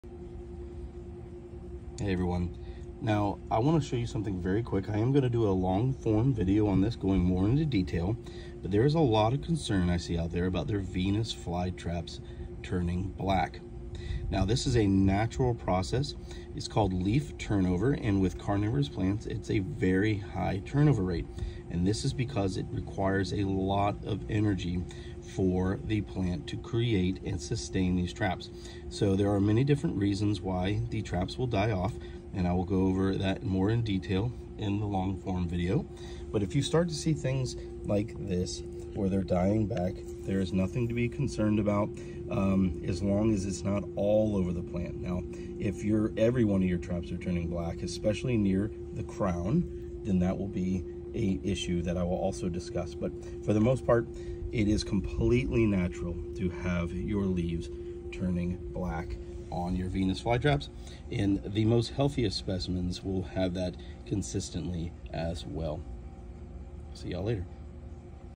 Venus Flytraps: Why do traps turn black?
A lot of folks get very concerned when their Venus flytraps start turning black. For the most part it is a natural process.